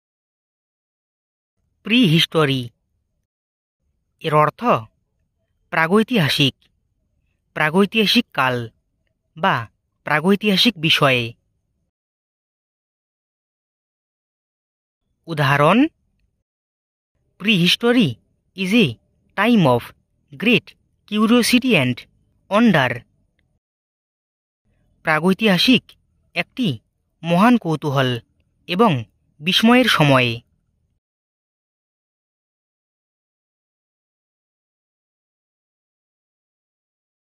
Prehistory Meaning in Bengali With Example || Prehistory শব্দটির বাংলা অর্থ কি হবে ?
এই ভিডিওতে prehistory শব্দটির বাংলা অর্থ নিয়ে আলোচনা করা হয়েছে। আশা করছি ভিডিওটি আপনাদের উপকারে আসবে।